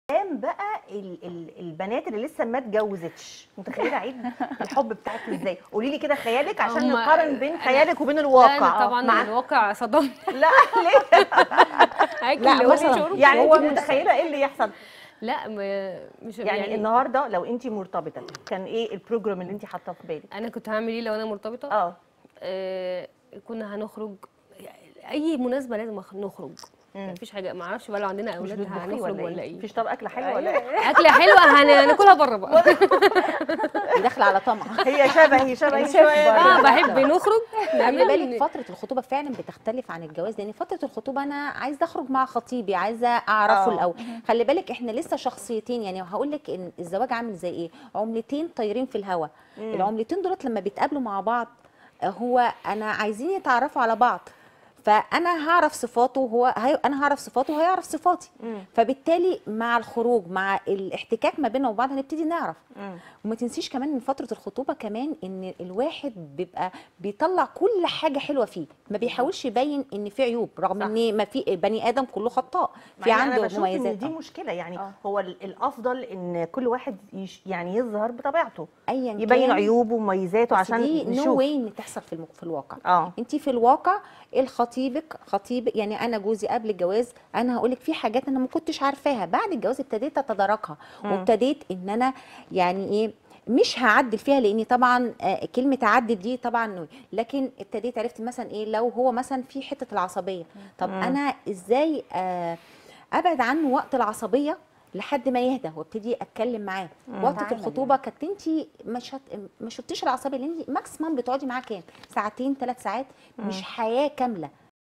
مطربة أنا خيالي في الحب كان واسع خالص وإتصدمت بالواقع
تقديم | الدكتورة والإعلامية/ أميرة الفيشاوي
وفي ضيافتها| د/ مروة محمود - خبيرة تنمية بشرية .. اليوتيوبر/ ريهام محمود .. المطربة / دينا خالد
لمتابعة الحلقة كاملة أدخل علي الرابط التالي:-

قناة_الصحة_والجمال# برامجنا